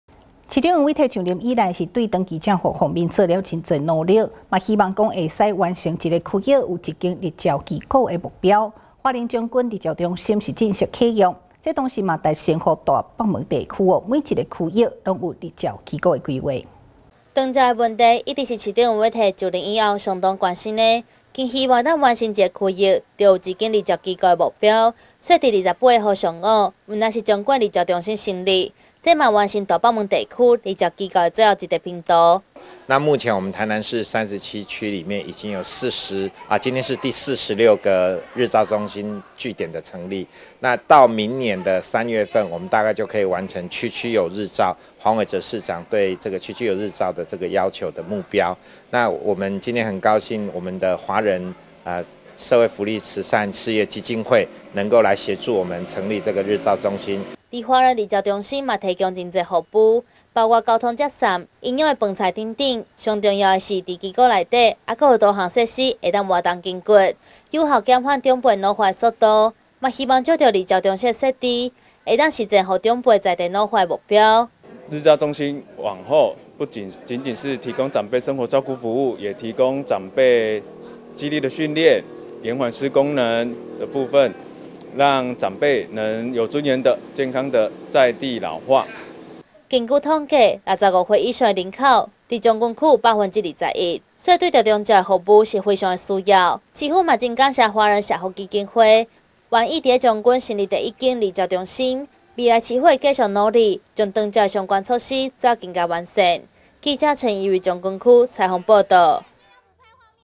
1081230新永安新聞-將軍日照啟用 完成大北門長照最後一拼圖
市長黃偉哲上任以後 對長照方面是做了許多努力 更希望可以完成一個區域 有一間日照機構的目標 而華仁將軍日照中心正式啟用囉 這同時也達成 讓大北門地區的每一個區域 都有日照機構的規劃